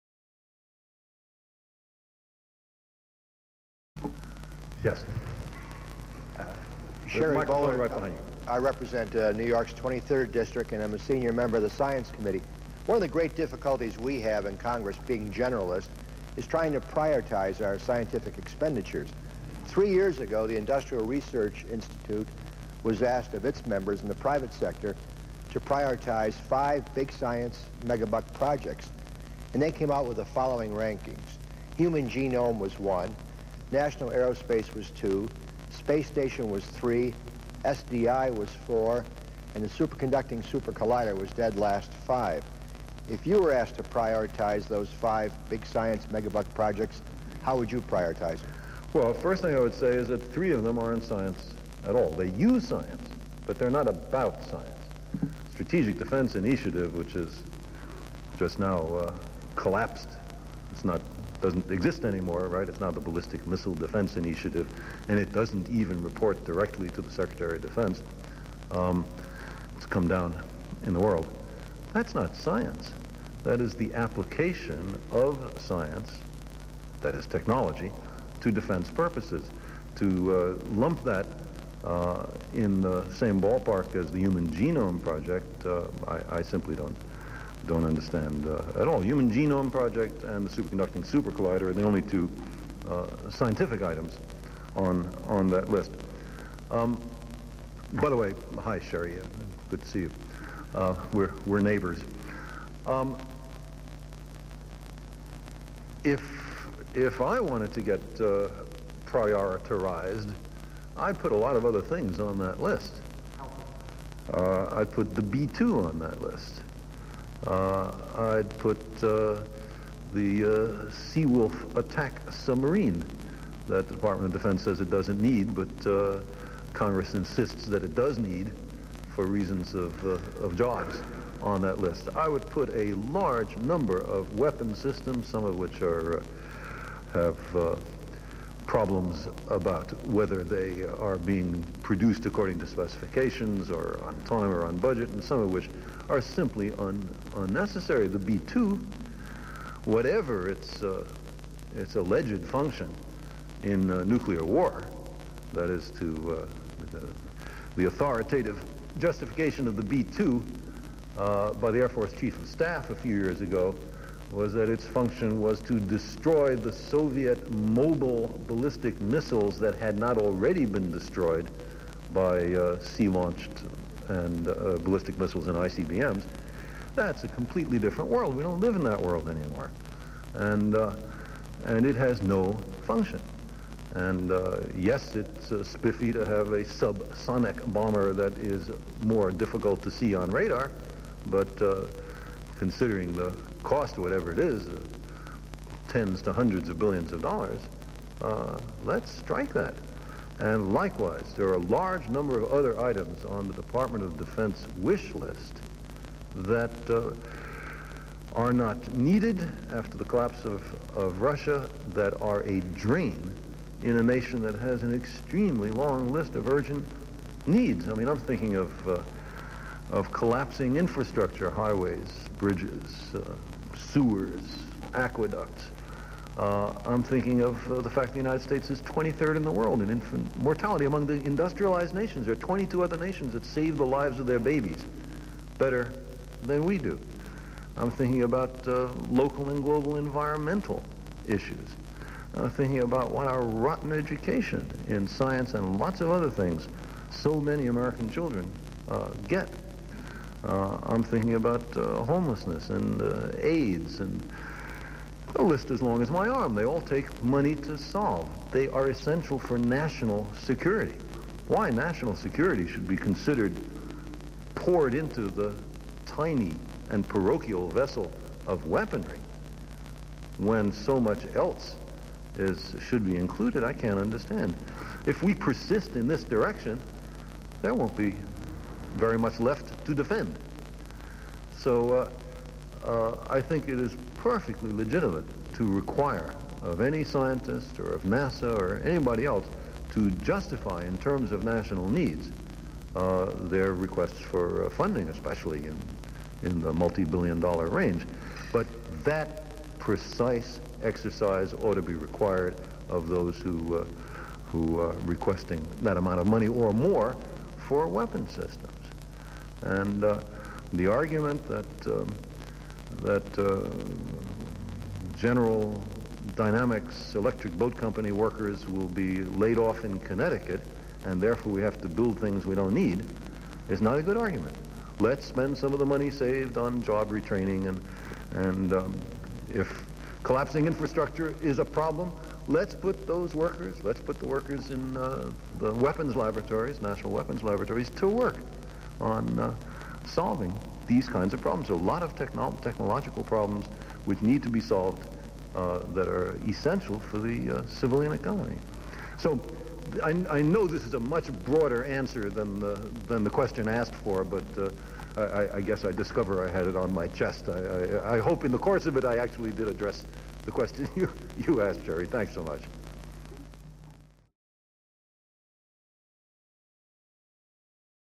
Carl Sagan discusses mega projects and large expenditures
The "state of the art" as of May 1993.